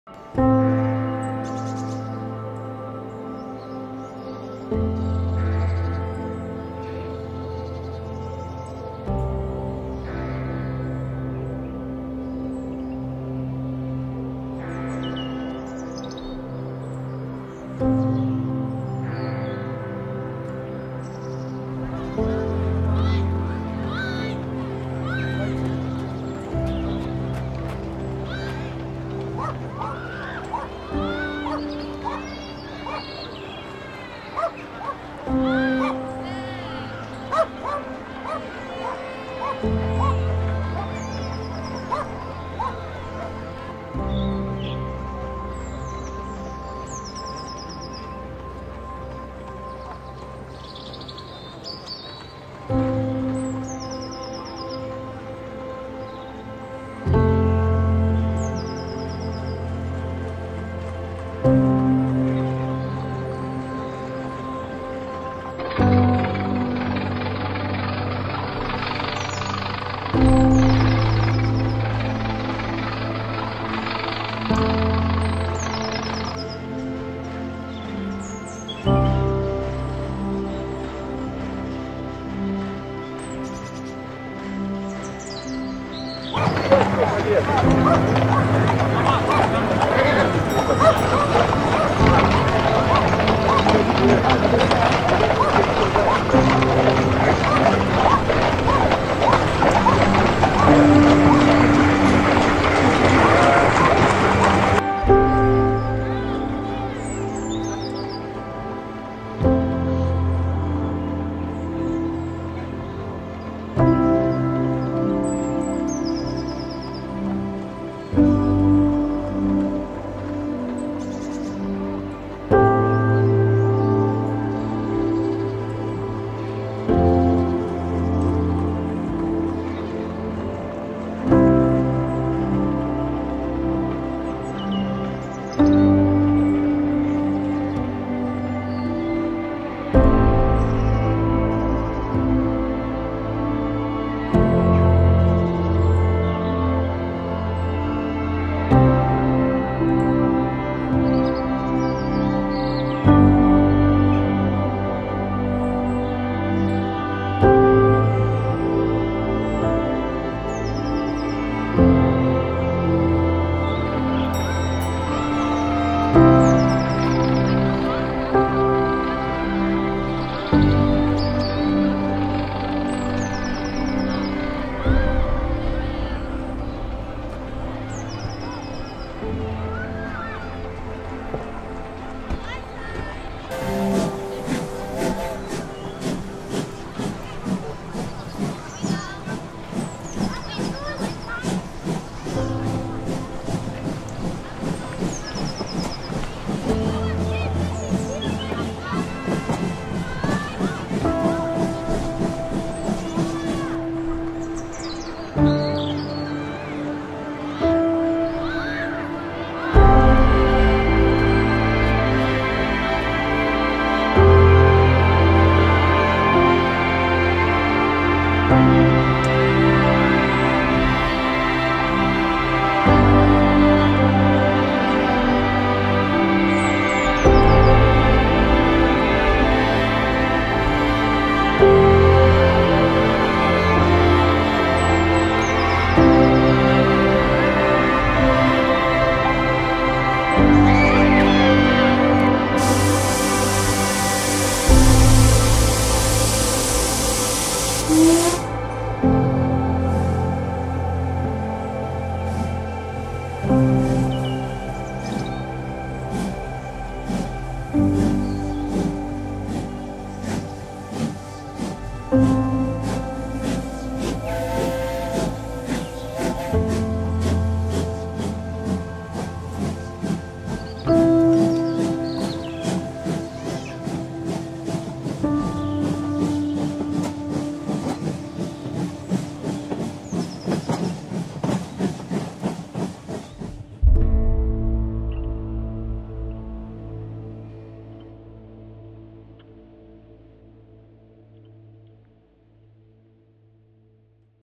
JAWORZNO - przedwojenne
Bardzo ciekawie i dobrze zmontowany oraz pokolorowany materiał
o przedwojennej zabudowie Jaworzna.
Pokolorowane zdjęcia, efekty dzwiękowe, ciekawa animacja.

Jaworzno - przedwojenne - kolorowe zdjęcia.
Pre-war photos from Jaworzno (Poland)